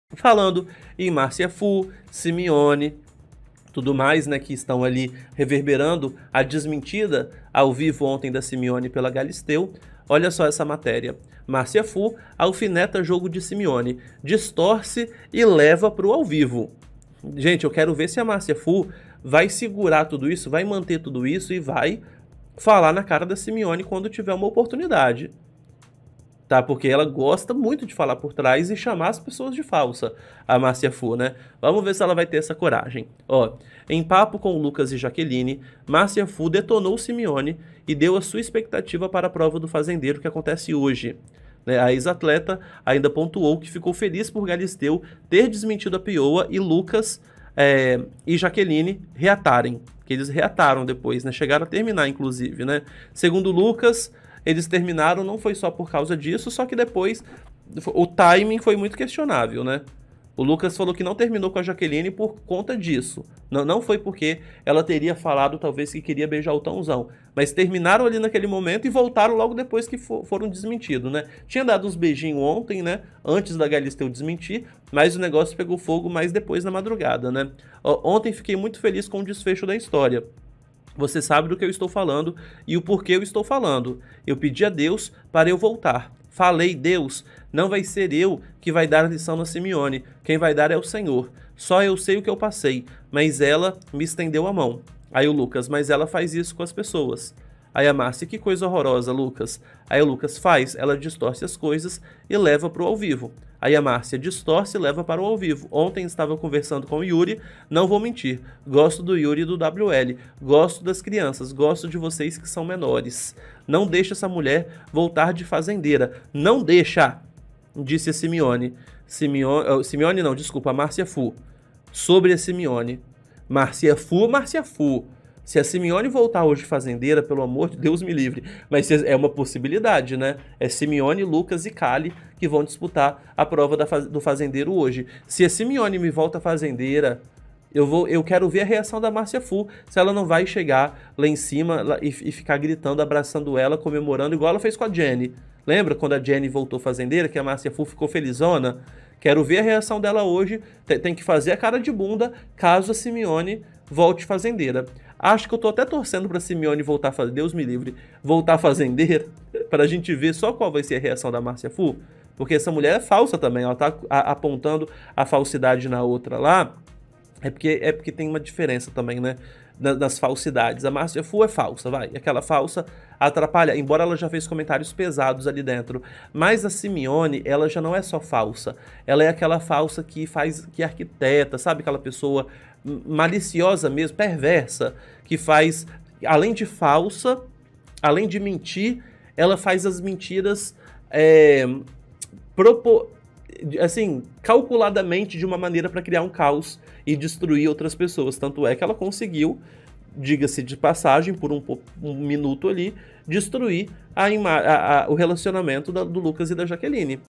Márcia Fu acusa peoa de distorcer falas e levar pro programa ao vivo | A Fazenda 2023
Corte da Live - Reality Show AFazenda15 (A Fazenda 2023) comentado AO VIVO em live interativa no CANAL DO ADAUTO.

📬 Envie presentes para o canal:
A/C: Adauto 🎁